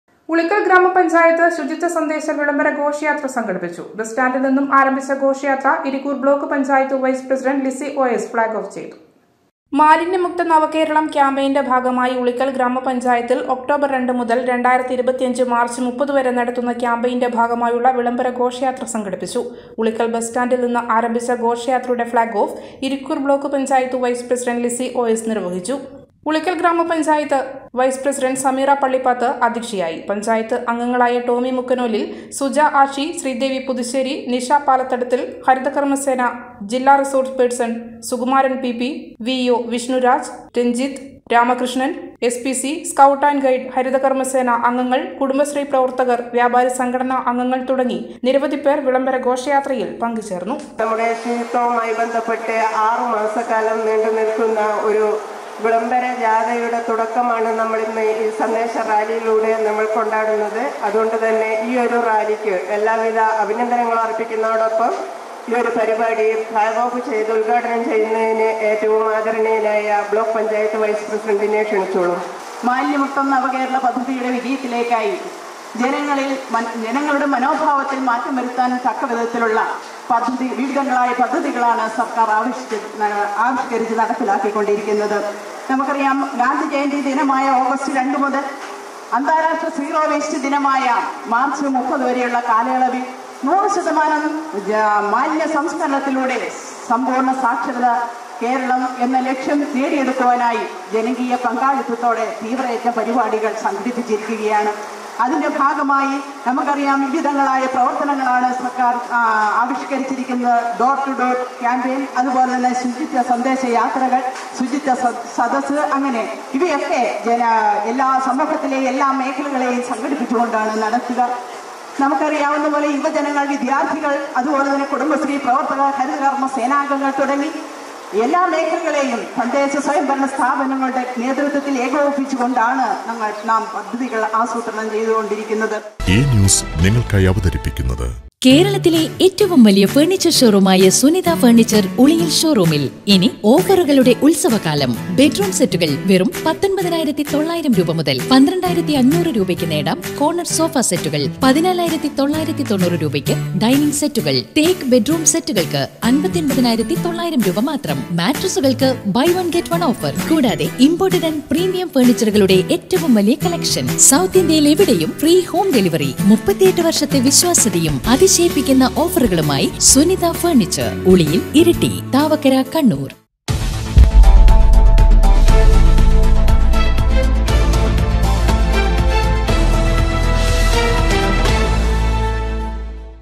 ഉളിക്കല്‍ ഗ്രാമപഞ്ചായത്ത് ശുചിത്വ സന്ദേശ വിളംബരഘോഷയാത്ര സംഘടിപ്പിച്ചു
ഉളിക്കല്‍ ഗ്രാമപഞ്ചായത്ത്  ശുചിത്വ സന്ദേശ വിളംബരഘോഷയാത്ര സംഘടിപ്പിച്ചു... ബസ്റ്റാന്‍ഡില്‍  നിന്നും  ആരംഭിച്ച ഘോഷയാത്ര ഇരിക്കൂര്‍ ബ്ലോക്ക്  പഞ്ചായത്ത്  വൈസ്  പ്രസിഡണ്ട്  ലിസി ഒ.എസ് ഫ്‌ളാഗോഫ് ചെയ്തു..